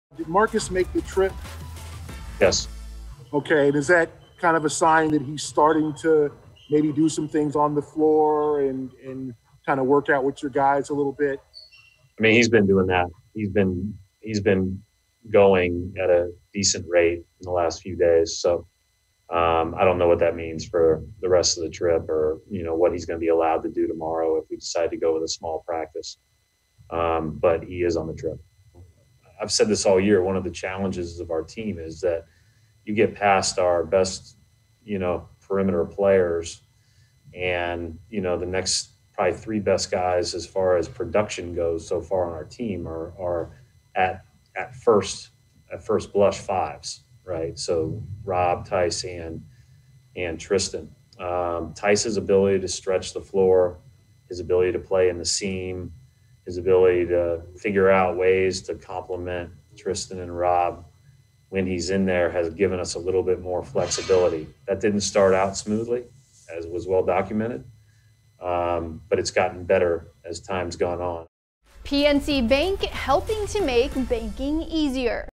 Brad Stevens Shares A Marcus Smart Injury Update, Discusses Big Men
Boston Celtics head coach Brad Stevens provides an injury update on Marcus Smart. Smart has not played since January 30.